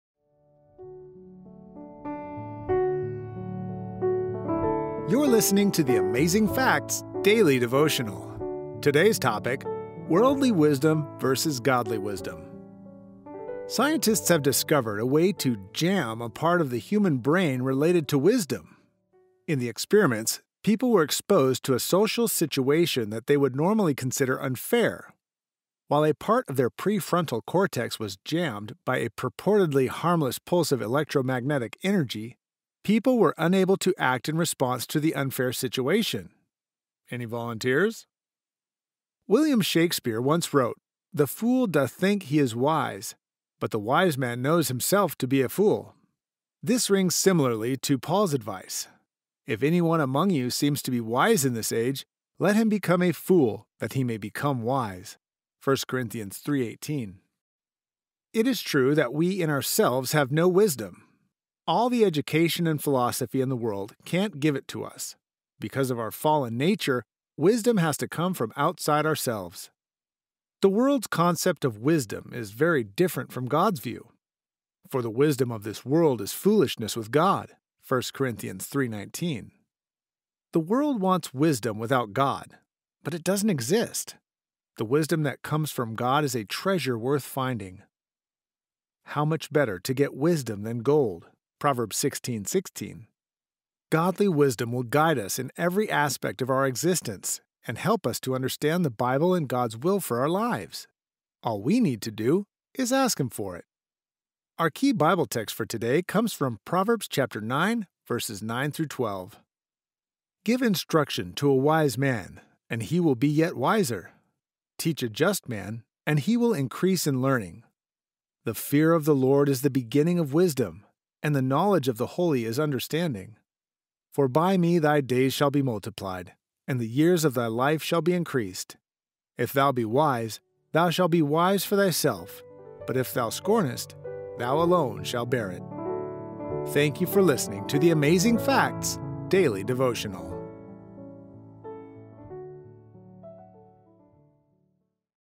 Worldly Wisdom vs God's Wisdom - Amazing Facts Daily Devotional (Audio only)
Amazing Facts Daily Devotional
"Worldly Wisdom vs God's Wisdom"

KEY BIBLE TEXTS
"Give instruction to a wise man, and he will be yet wiser: teach a just man, and he will increase in learning."
"The fear of the LORD is the beginning of wisdom: and the knowledge of the holy is understanding."
"For by me thy days shall be multiplied, and the years of thy life shall be increased."
"If thou be wise, thou shalt be wise for thyself: but if thou scornest, thou alone shalt bear it."
Proverbs 9:9-12